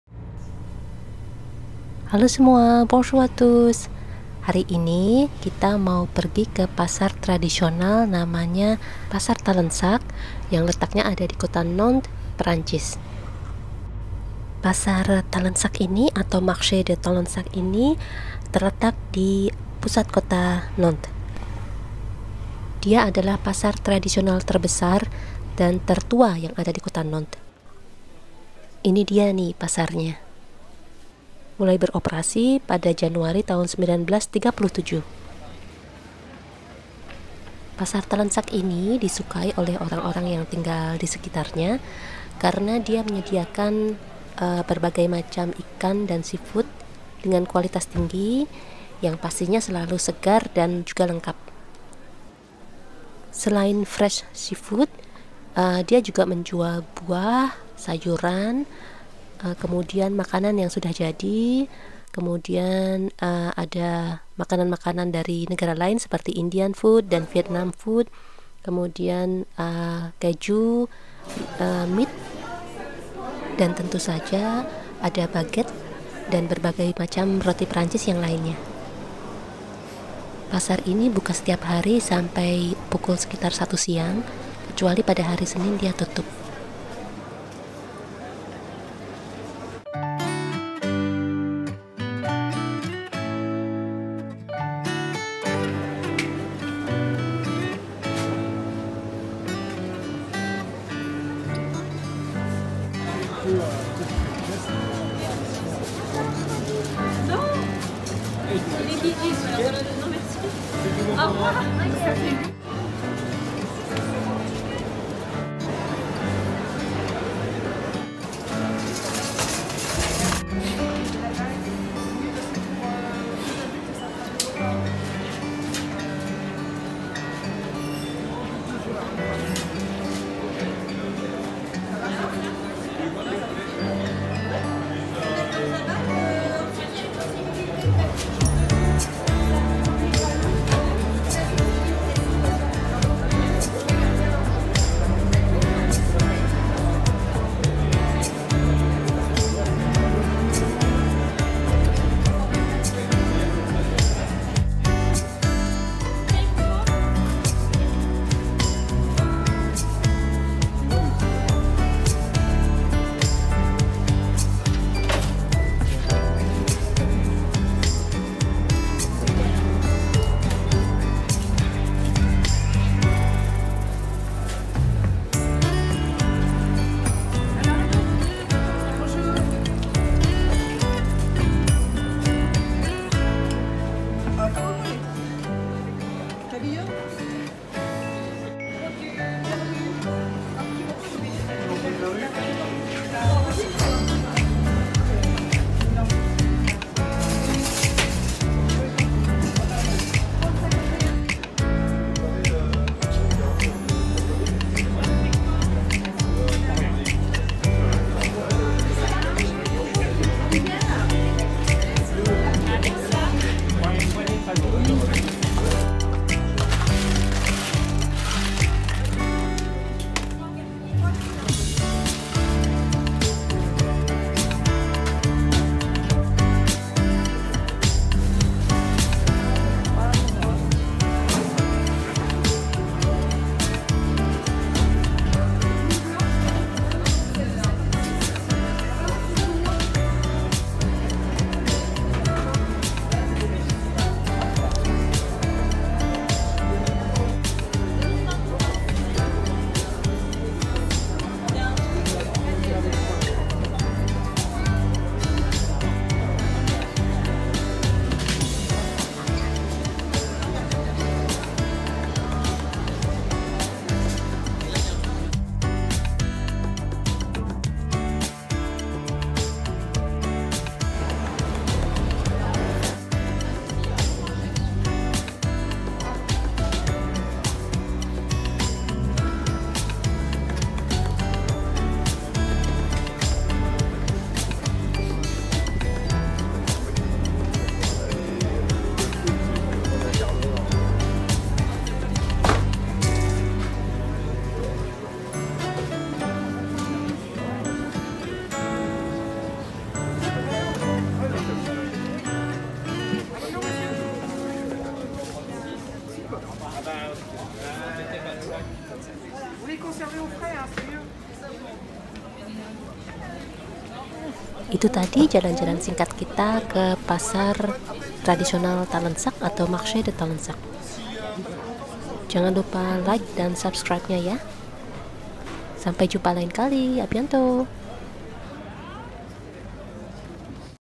Talensac Traditional Market Nantes France / Marché de Talensac / Pasar Tertua dan terbesar di Nantes
Talensac market or Marché de Talensac, is the oldest and biggest traditional market in Nantes, France.
Famous with high quality fresh fish and seafood, this market operate everyday except Monday.
Beside fresh seafood, their pâtisserie are also famous with their delicious bread and cake. You can easily see people queueing in front of the cake shop, to buy fresh traditional baguette, pain aux raisin, or gâteau au chocolat.
A must to visit if you want to feel the real Nantais morning.